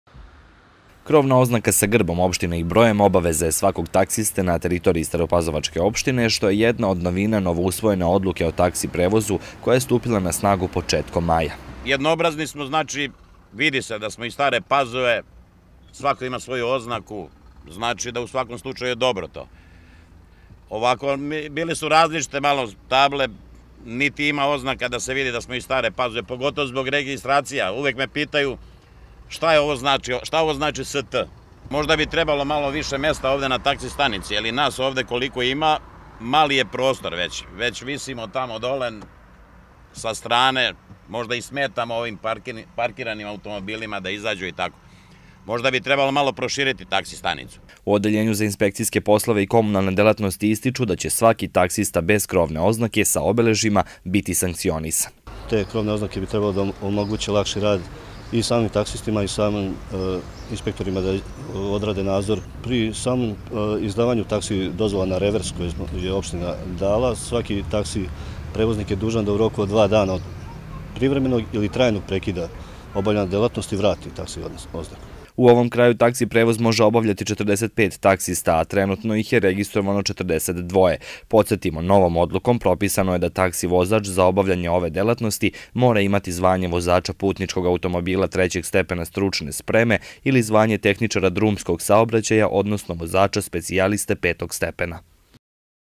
Taksi prevoznici sa krovnim oznakama
Od početka maja u opštini Stara Pazova na snazi je nova Odluka o taksi prevozu kojom su propisani bliži uslovi za obavljanje ove vrste usluge, karakteristike i obeležja taksi vozila i način obavljanja. U toku je dodela krovnih oznaka koje mora posedovati svaki taksista.